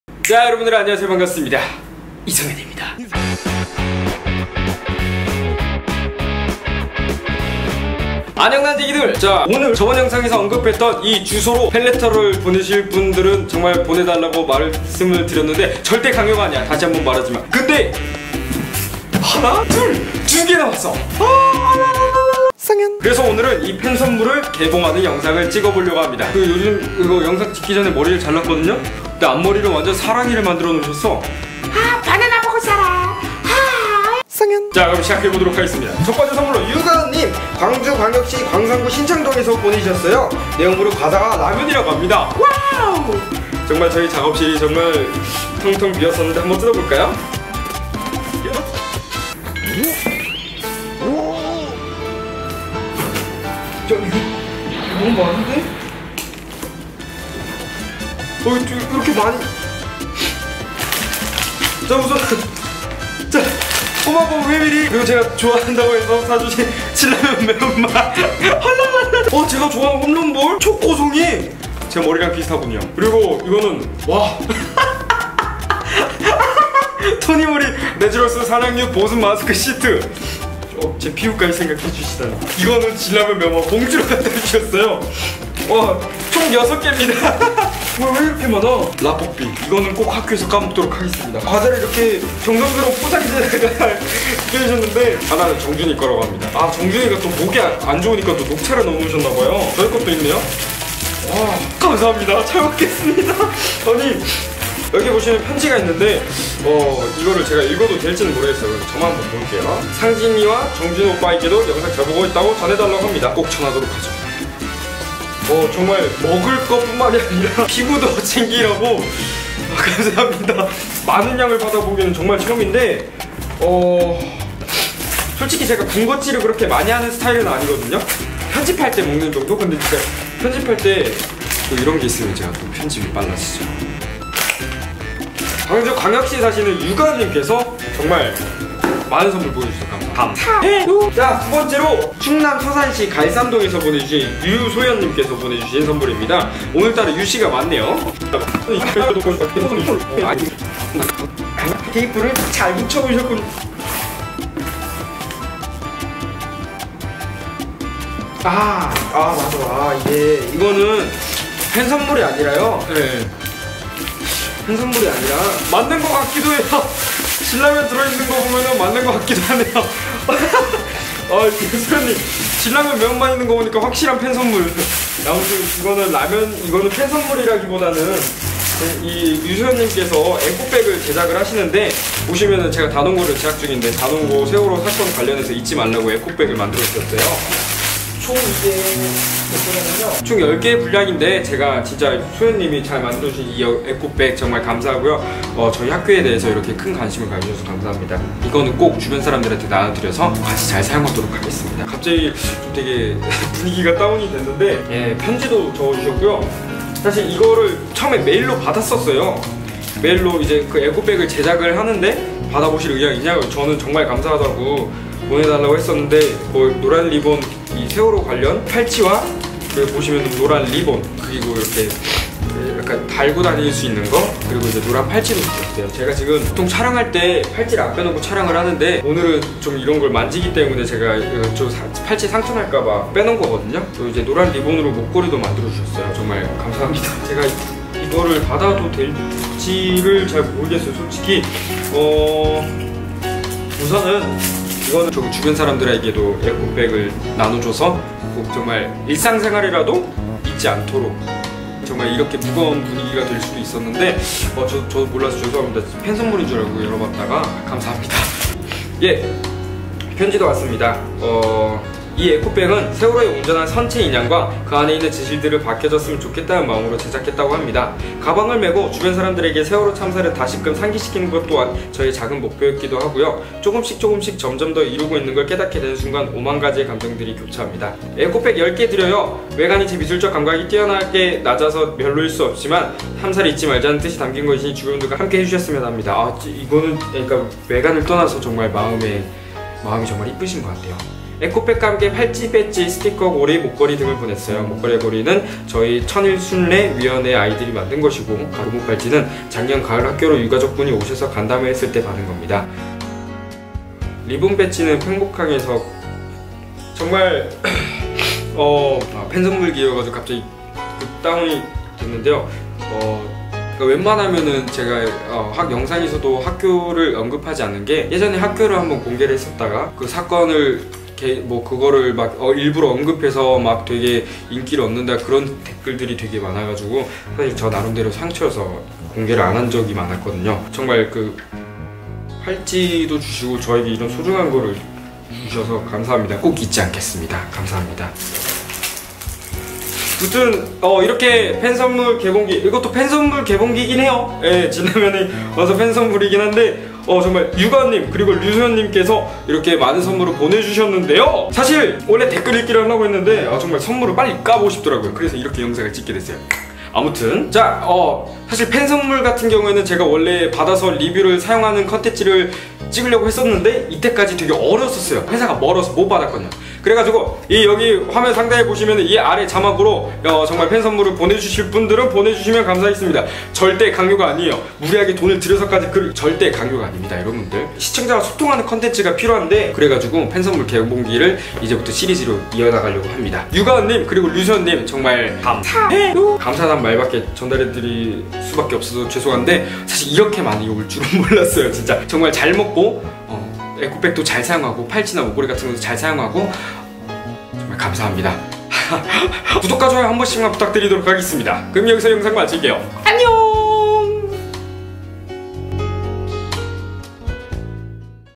첫번째 선물 개봉기! [나에게도 드디어 팬선물이!] - 정말 감동쏟은 선물개봉기 1탄
유튜브 전용 컨텐츠는 매주 목요일,일요일 오후 6시에 영상이 업로드 됩니다!

흐어유ㅠㅠ 저에게도 선물개봉기를 찍는 날이 올줄 몰랐네요
항상 과분한 사랑 감사합니다.
여러분들의 호응과 응원이 있어서 요즘들어 부쩍 힘이 나는 것 같아요!
항상 감사하고 사랑합니다 :)

유가은님 , 류소연님 정말 감사합니다!